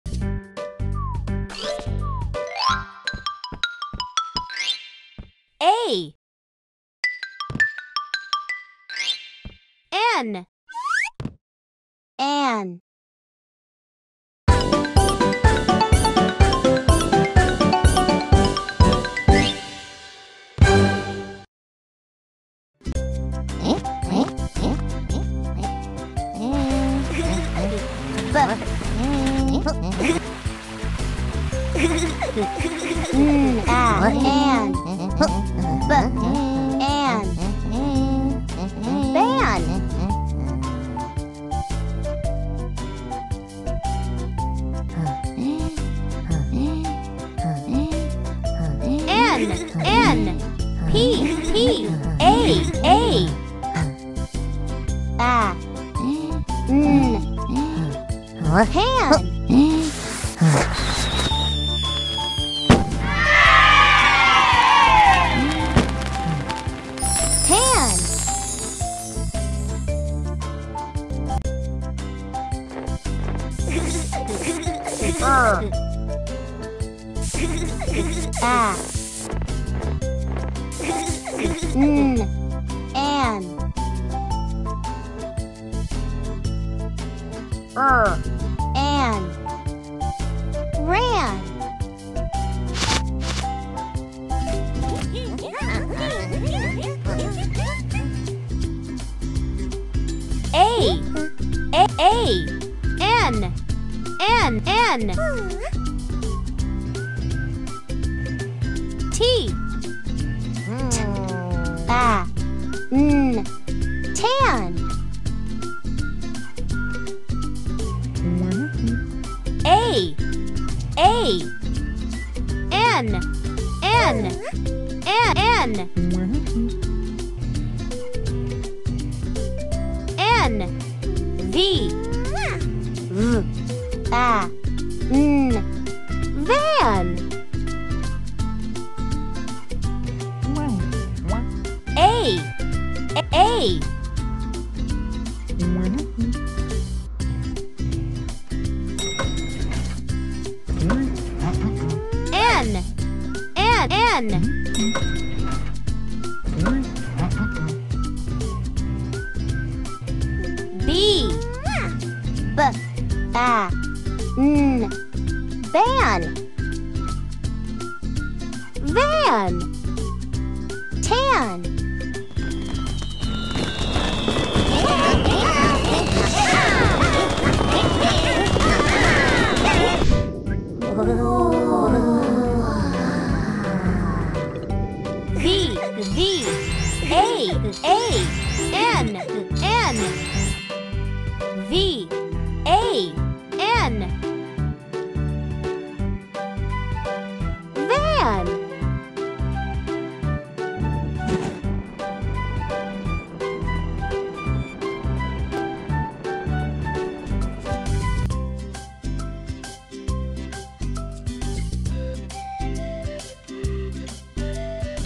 Phonics
This animated video helps children learn AN sound in English. sound. In this video, your kids will practice phonics /AN/ sound in the next words: ban, pan, ran, tan, van, van.

You will be amazed at how easily your little one can learn phonics!